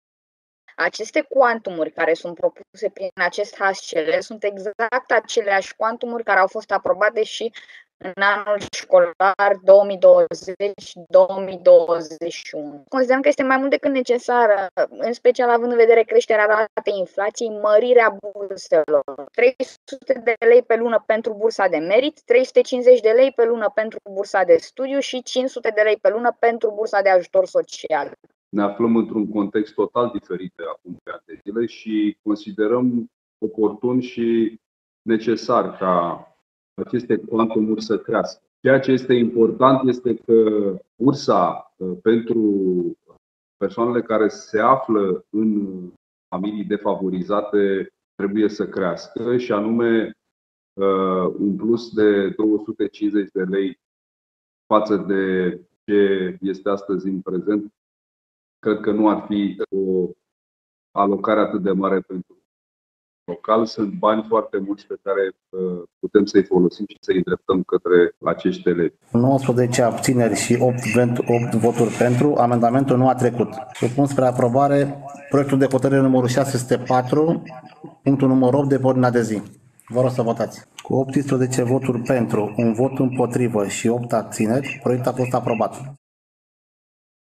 Elevii au cerut mărirea burselor
De cealaltă parte, reprezentanții Asociației Elevilor din Constanța au solicitat astăzi în cadrul ședinței de Consiliu Local mărirea burselor. Aceștia au susținut că ele au rămas neschimbate, în ciuda creșterii inflației. Elevii au cerut ca măcar bursa socială să crească. Și unii consilieri au venit cu un amendament și au propus ca această bursă să se mărească la 500 de lei pe lună. Însă, amendamentul nu a trecut, în schimb proiectul inițial, da. Concret, elevii vor fi primii 1.000 de lei pentru bursa de performanță, 200 de lei pentru cea de merit și câte 250 de lei pentru studiu și ajutor social.